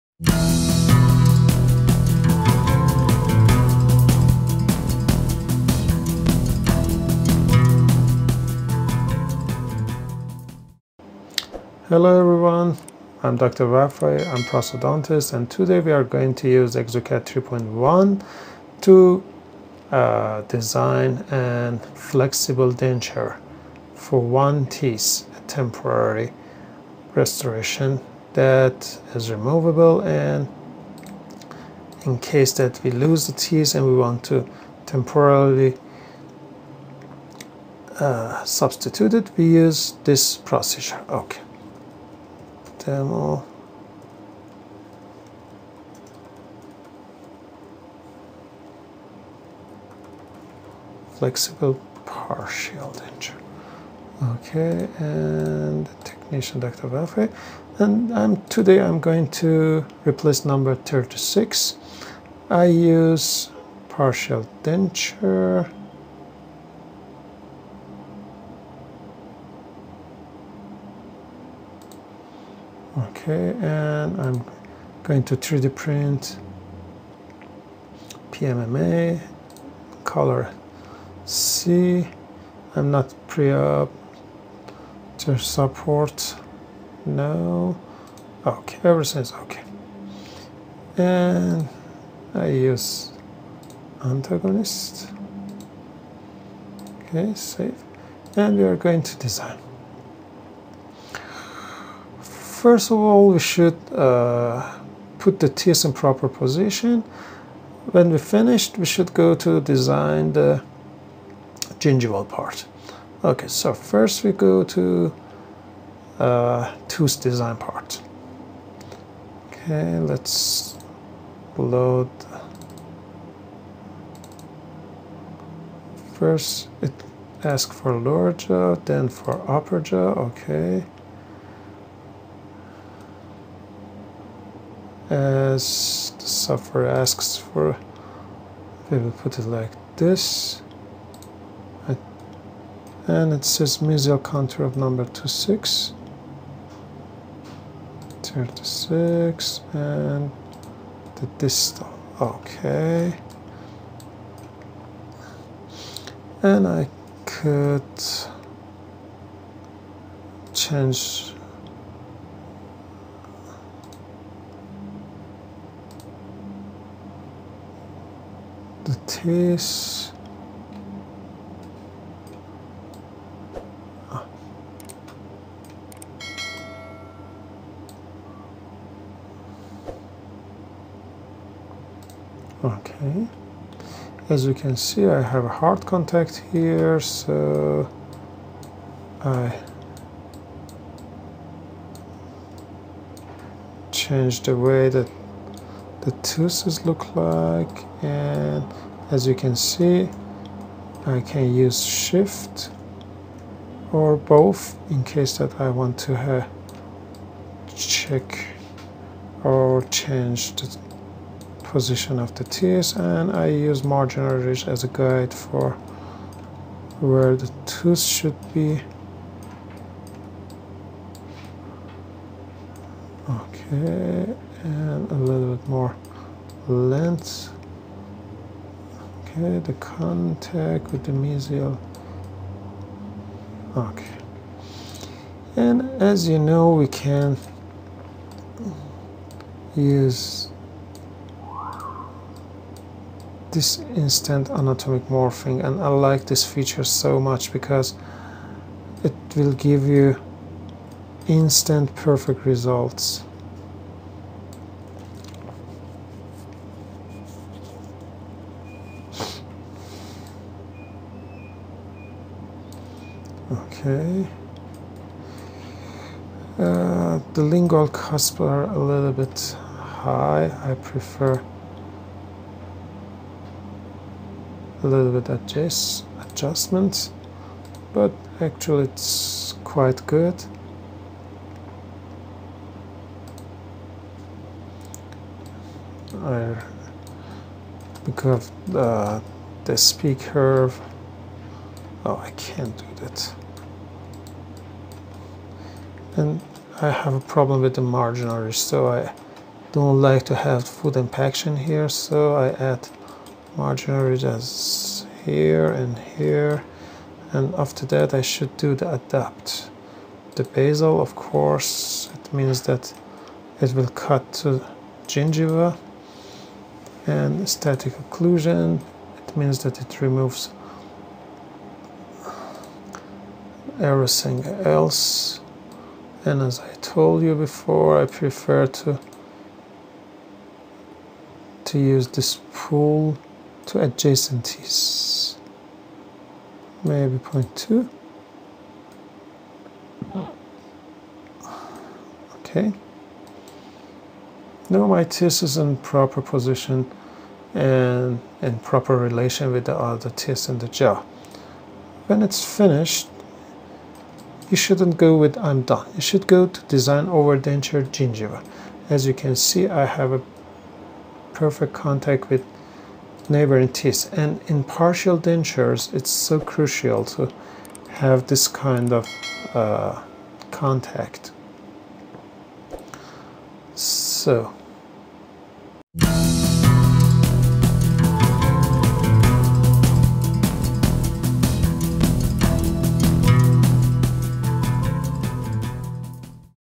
how to design flexible partial denture in exocad3.1rijka(part1)
this tutorial video teach you how to design on flexible partial denture in exocad 3.1 ridjika
Dental Digital excellence
▪️Making 3D printing the new standard for surgical excellence.
▪️Customized 3D models and surgical instruments.